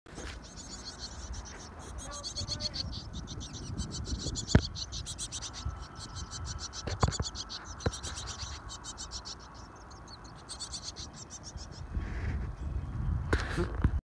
Carolina Chickadee Babies 😊❤️
This is about 10 years I’ve been keeping track of the chickadee population in my yard. Every year they have young. They’re back again.